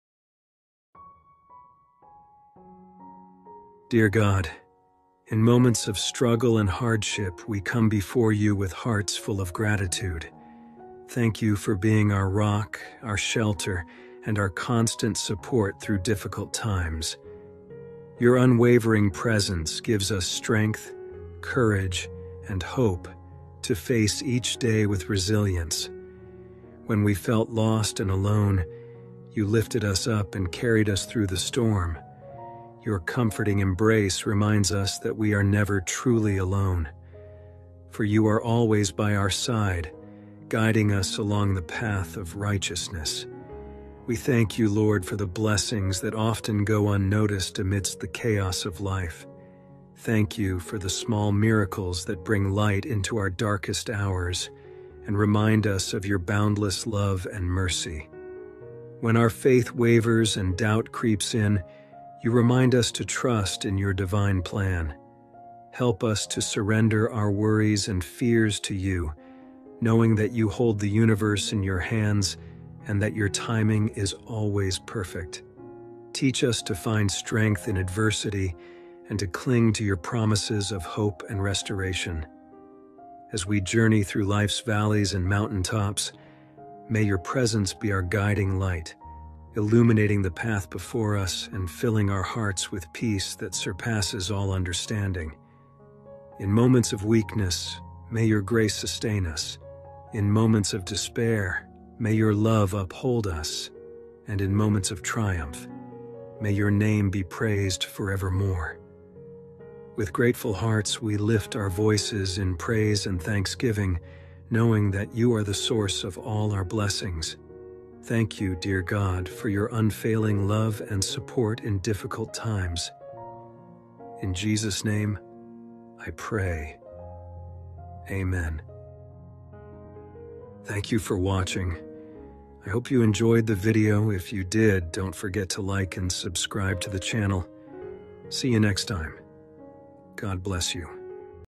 Lord, Your comforting embrace reminds us that we are never truly alone, for You are always...
Dear God, In moments of struggle and hardship, we come before You with hearts full of gratitude. Thank You for being our rock, our shelter, and our constant support through difficult times.

Your unwavering presence gives us strength, courage, and hope to face each day with resilience. When we felt lost and alone, You lifted us up and carried us through the storm.

Your comforting embrace reminds us that we are never truly alone, for You are always by our side, guiding us along the path of righteousness. We thank You, Lord, for the blessings that often go unnoticed amidst the chaos of life.

Thank You for the small miracles that bring light into our darkest hours and remind us of Your boundless love and mercy. When our faith wavers and doubt creeps in, You remind us to trust in Your divine plan.

Help us to surrender our worries and fears to You, knowing that You hold the universe in Your hands and that Your timing is always perfect. Teach us to find strength in adversity and to cling to Your promises of hope and restoration.

As we journey through life's valleys and mountaintops, may Your presence be our guiding light, illuminating the path before us and filling our hearts with peace that surpasses all understanding.

In moments of weakness, may Your grace sustain us. In moments of despair, may Your love uphold us. And in moments of triumph, may Your name be praised forevermore.

With grateful hearts, we lift our voices in praise and thanksgiving, knowing that You are the source of all our blessings. Thank You, dear God, for Your unfailing love and support in difficult times. Amen

Let's pray together morning prayer
powerful prayer
Short prayer to god
night prayer
evening prayer
catholic